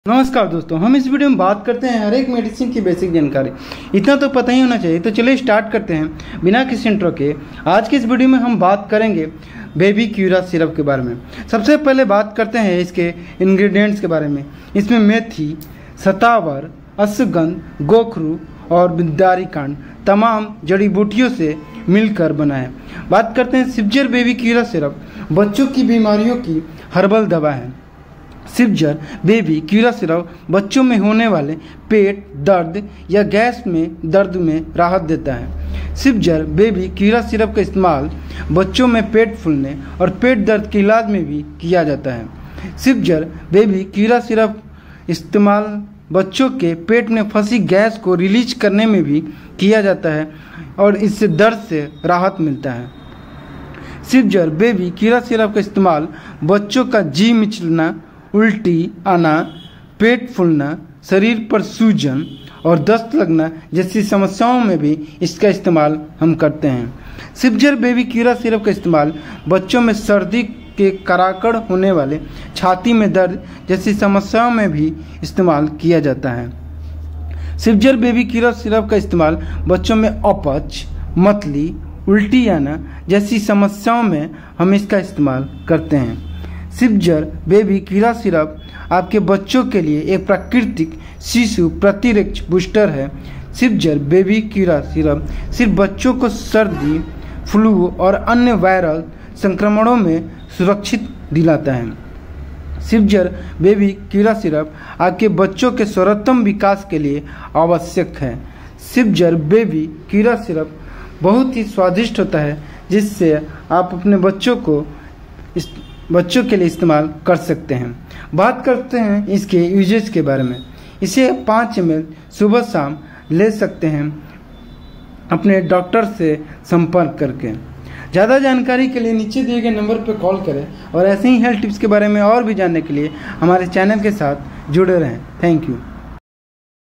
Cipzer Baby cura syrup in hindi|| uses , benifits || cipzer Baby cura सिरप के फायदे
BABY CURA Syrup
सिप्जर बेबी क्यूरा सिरप बच्चों की बीमारियों की हर्बल दवा है।
सिप्जर बेबी क्यूरा सिरप बच्चों में होने वाले पेट दर्द या गैस के दर्द में राहत देता है
सिप्जर बेबी क्यूरा सिरप का इस्तेमाल बच्चों में पेट फूलने और पेट दर्द के इलाज में भी किया जाता है।
बेबी क्यूरा का इस्तेमाल बच्चों में पेट से जुड़ी समस्याओं जैसे अपच और पेट के  ऐंठन के इलाज में भी किया जाता है
सिप्जर बेबी क्यूरा सिरप का  इस्तेमाल बच्चों के पेट में फंसी गैस को रिलीज करने मै भी किया जाता है और दर्द से राहत मिलती है।
सिप्जर बेबी क्यूरा सिरप का इस्तेमाल बच्चों में बार-बार भूख लगना,बिना किसी कारण के वजन कम होना,या जीभ सफेद होना, मुंह से बदबू आना जैसी समस्याओ में भी किया जाता है।
सिप्जर बेबी क्यूरा सिरप का इस्तेमाल बच्चों का  जी मिचलाना,उल्टी आना,पेट फूलना,शरीर पर सूजन आना और दस्त लगना जैसी समस्याओ में भी किया जाता है।
सिप्जर बेबी क्यूरा सिरप का इस्तेमाल बच्चों के पेट में कीड़े होने की समस्या में भी किया जाता है
सिप्जर बेबी क्यूरा सिरप का इस्तेमाल  बच्चो के शारीरिक विकास का रुक जाने की जैसी समस्या में भी किया जाता है
सिप्जर बेबी क्यूरा सिरप का इस्तेमाल बच्चो मै सर्दी के कारड़ होने वाले  छाती में दर्द जैसी समस्या में भी किया जाता है।
सिप्जर बेबी क्यूरा सिरप का इस्तेमाल बच्चो मै अपच,पेट दर्द,मतली,उल्टीं और पेट फूलने जैसे लक्षण को दूर करता हैं। बच्चों की इस प्रॉब्लम को भी बेबी क्यूरा से ठीक किया जाता है।
सिप्ज़र बेबी क्यूरा सिरप बच्चो मै इम्युनिटी बूस्टर का काम करता है
सिप्ज़र बेबी क्यूरा सिरप आपके बच्चों के लिए एक प्राकृतिक शिशु प्रतिरक्षा बूस्टर है। सिप्ज़र बेबी क्यूरा सिरप बच्चों को सर्दी,फ्लू और अन्य वायरल संक्रमणों से सुरक्षित रखता है
सिप्ज़र बेबी क्यूरा सिरप नवजात शिशुओं मै वायरल संक्रमणों से  देखभाल करने और  बच्चे के विकास में तेजी लाने के लिए भी सिप्ज़र बेबी क्यूरा सिरप इस्तेमाल किया जाता हैं।
सिप्ज़र बेबी क्यूरा सिरप  बच्चो मै आवश्यक विटामिन और पोषक तत्वों की कमी को पूरा करता है
सिप्ज़र बेबी क्यूरा सिरप आपके बच्चे के सर्वोत्तम विकास के लिए आवश्यक है।
सिप्ज़र बेबी क्यूरा सिरप बहुत स्वादिष्ट होता है और आपका बच्चा इस सिरप को पीना पसंद करता है
Ingredients :- Methi,shatavar,asgandh,gokhru,Bidari Qand,

Dosage:- 5 ML सुबह और शाम लें सकते है  या लेने से पहले अपने डॉक्टर से परामर्श ज़रूर ले